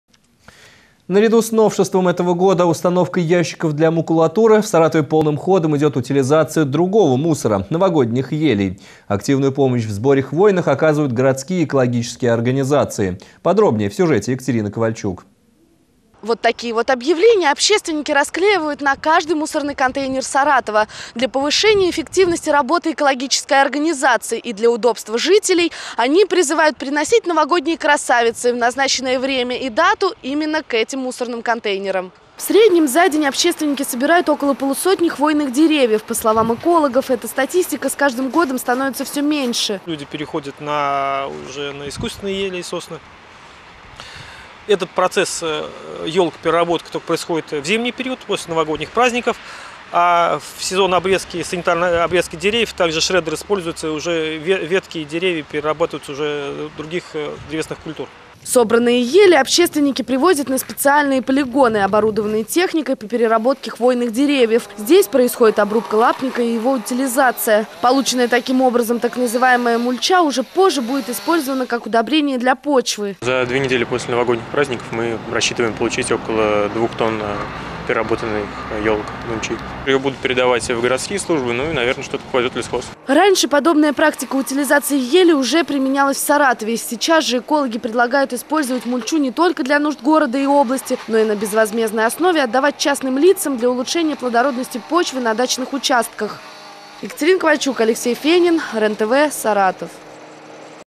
Экологические организации утилизируют прошлогодние елки
Наряду с новшеством этого года - установкой ящиков для макулатуры, в Саратове полным ходом идет утилизация другого мусора - новогодних елей. Активную помощь в сборе хвойных оказывают городские экологические организации. Подробнее в сюжете Екатерины Ковальчук от 15 января.